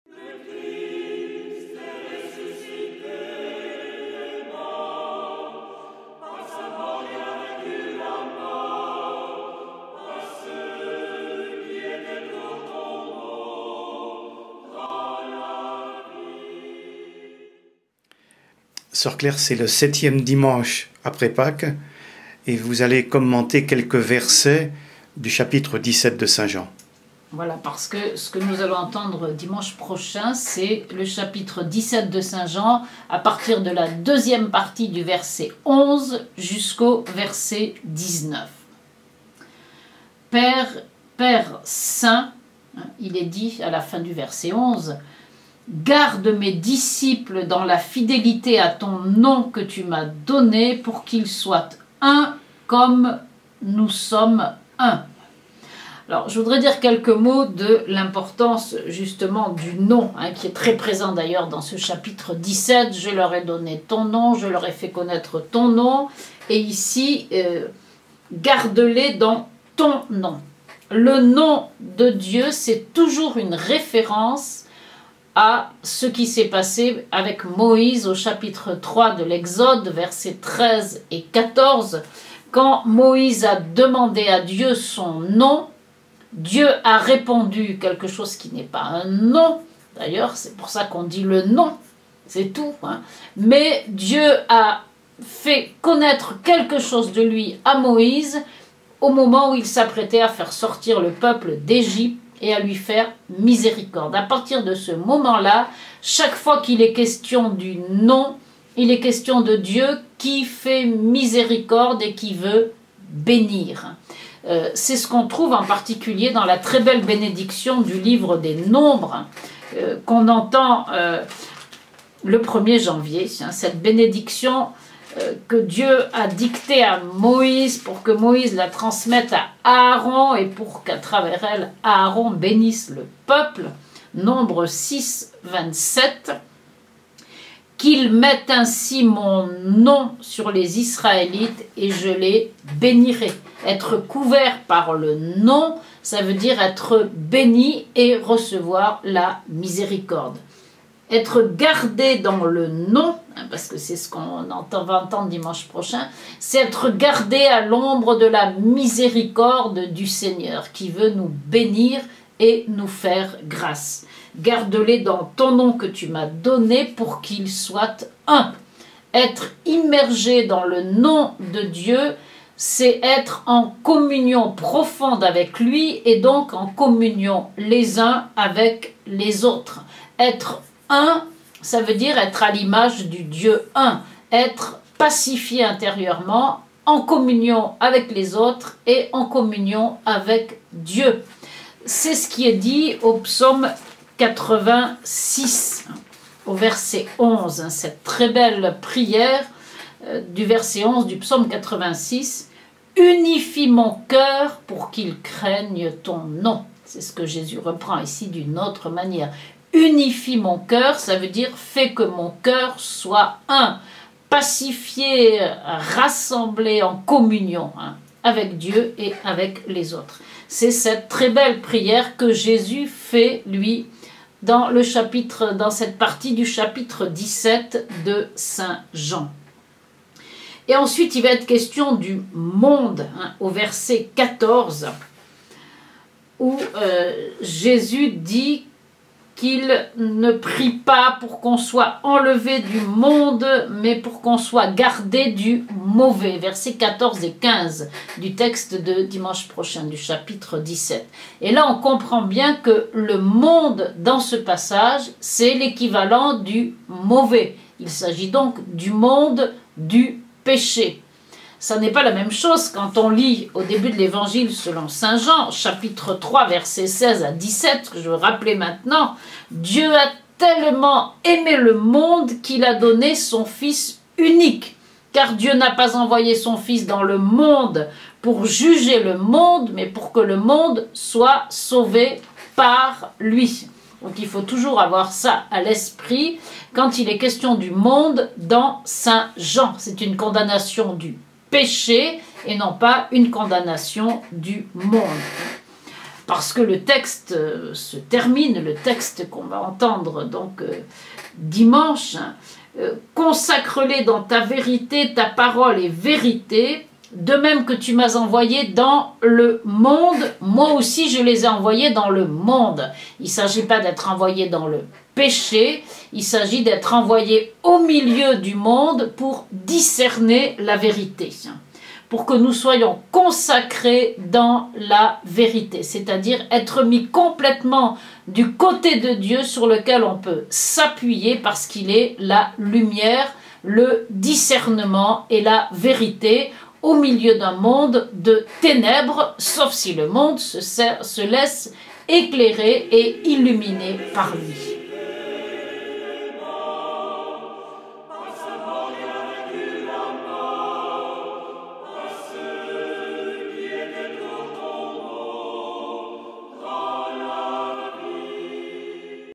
Garde mes disciples dans la fidélité à ton Nom. 7ème dimanche de Pâques
Commentaire de l'évangile selon saint Jean 17,11b-19